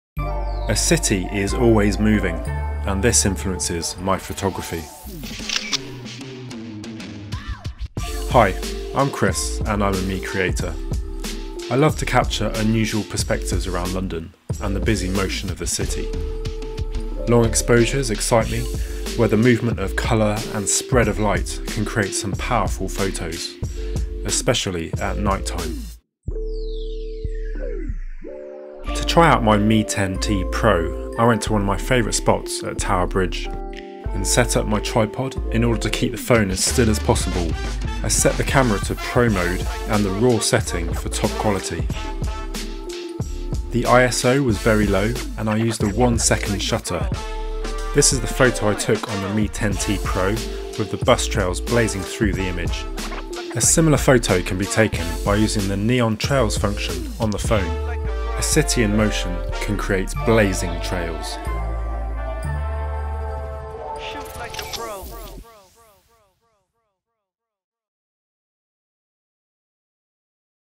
Xiaomi UK | ShootLikeAPro: How to shoot light trails on Mi 10T Pro
ShootLikeAPro Lesson 3: What do you miss the most about city life? Chris Dalton, London-based photographer and architect, gives us a lesson on how to shoot neon trails with Mi 10T Pro.

Theme song by Luis Guanzon (IG: @luis_guanzon).

Mi 10T Pro 5G is available online and at Vodafone UK.